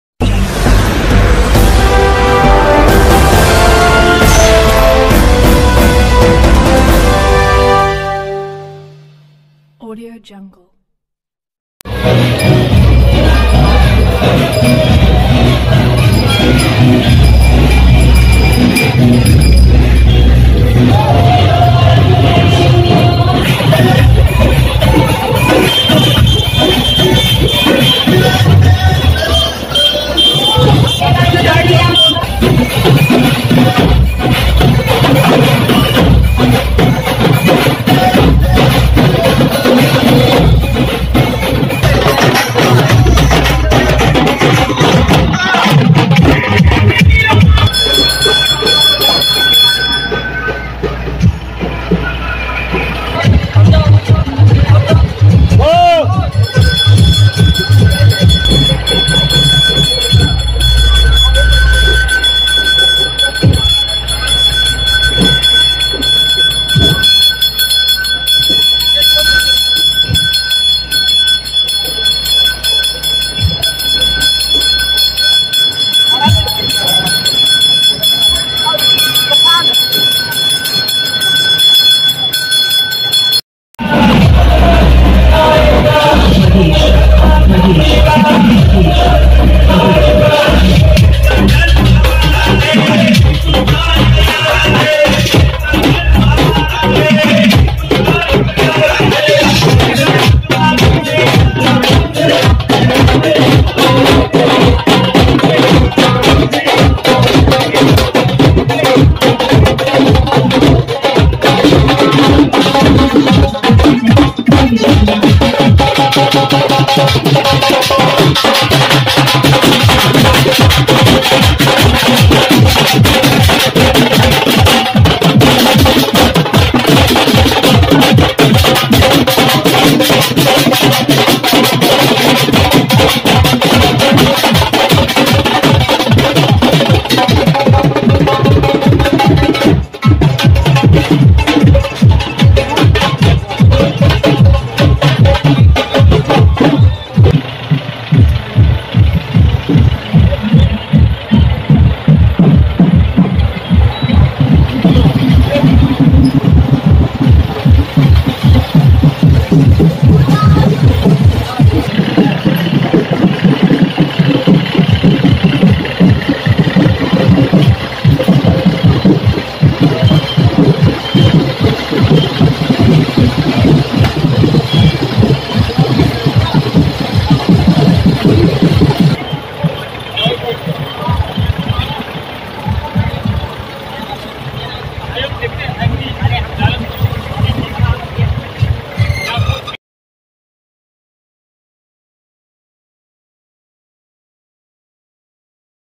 अयोध्या के मंदिरों से निकला जगन्नाथ भगवान जी की रथयात्रा।
Video from महताब अहमद